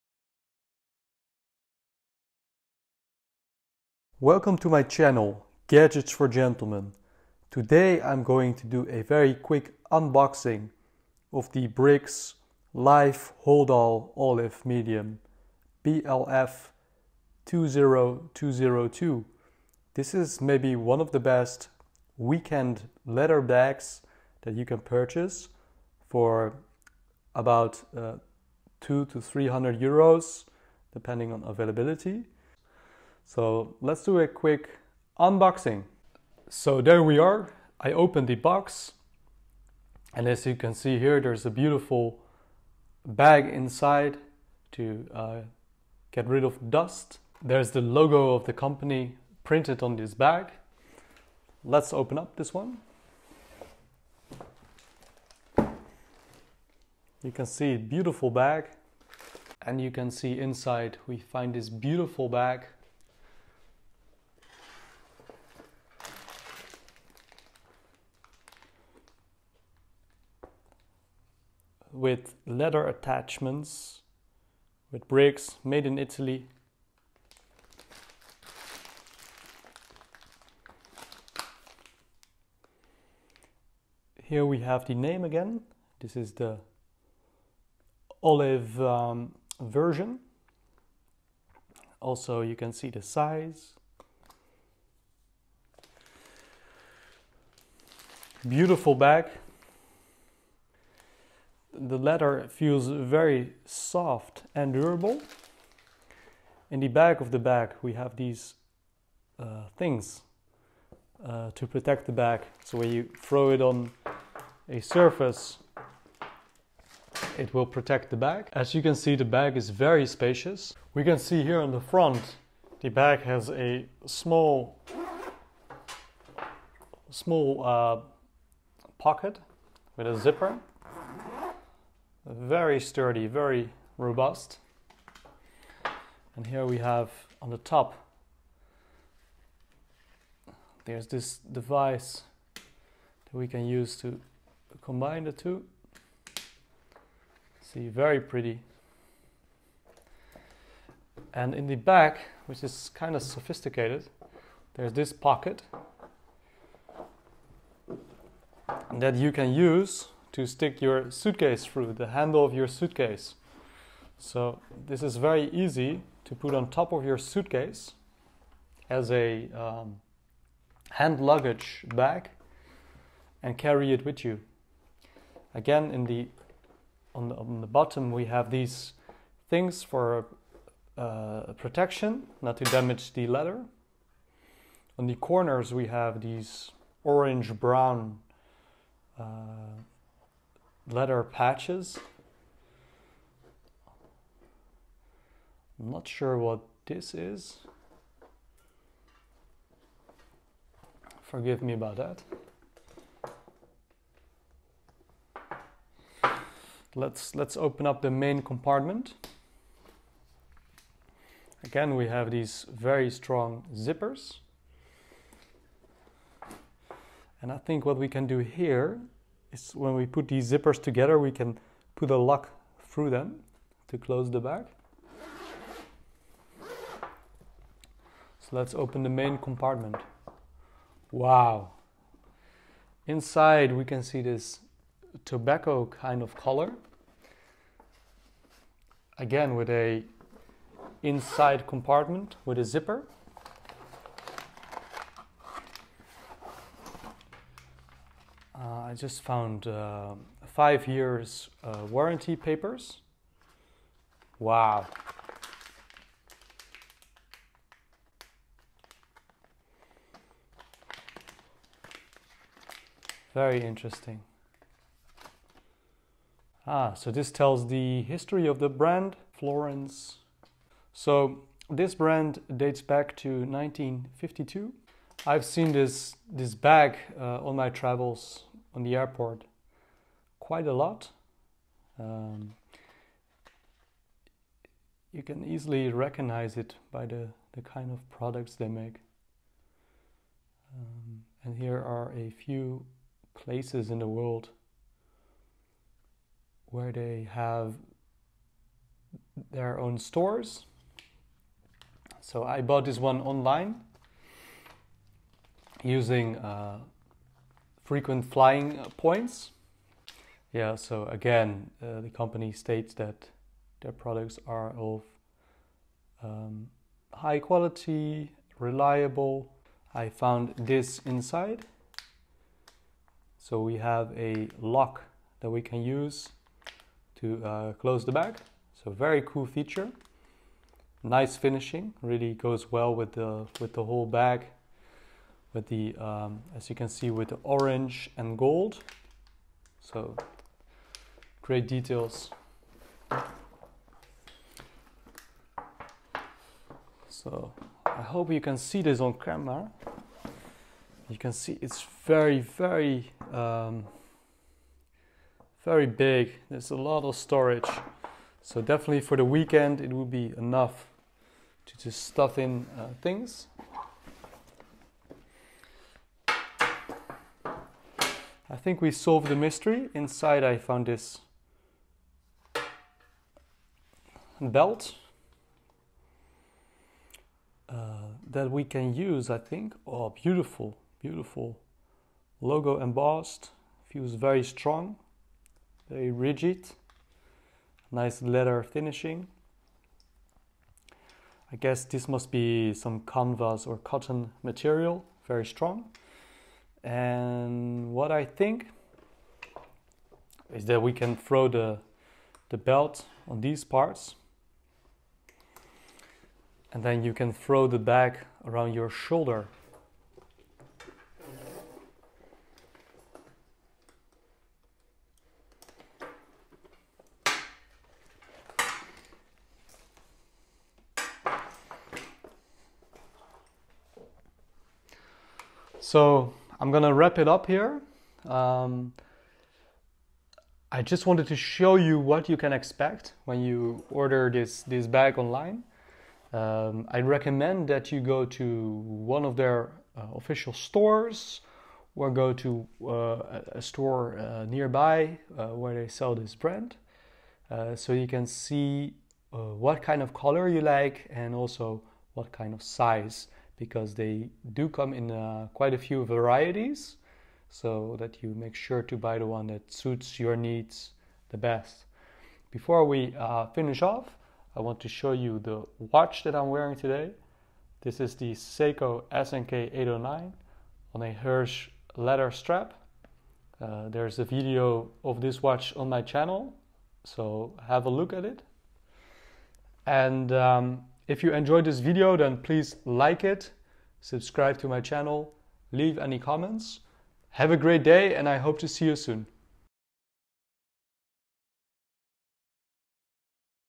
The Best And Most Stylish Carry-On Luggage Weekend Bag | BRIC’S LIFE HOLDALL OLIVE 22 Inch Unboxing
In this short video I unbox the Bric's Life Holdall weekend bag. This bag can be found here: